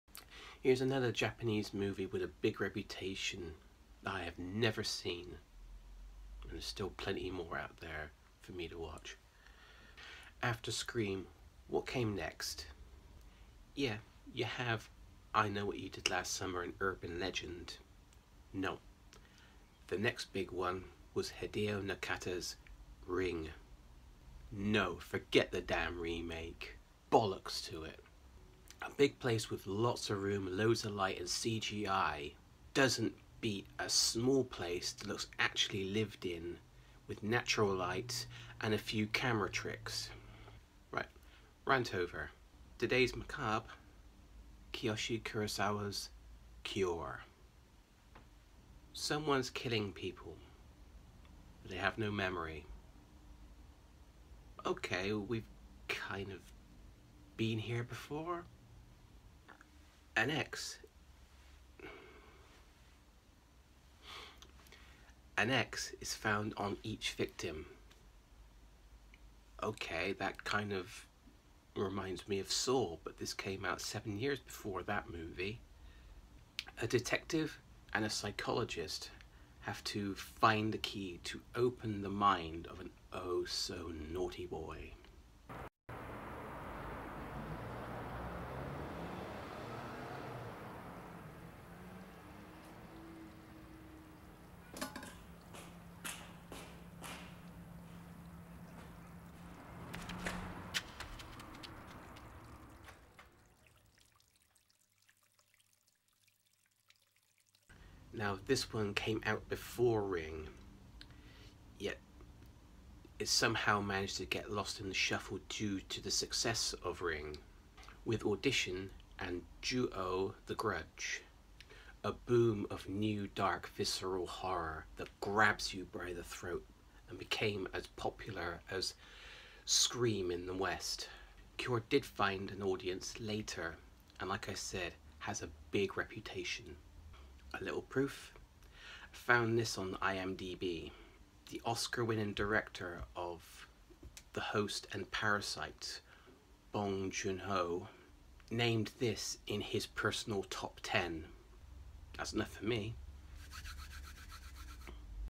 Halloween 2021: Cure (1997) Introduction
Day thirteen and another trip to Japan for one of their best of 1990s macabre
I know I look very red-eyed. My hayfever's playing up

Cure (1997)
Directed by Kiyoshi Kurosawa
From Daiei Studios, Twins Japan & Eureka The Masters of Cinema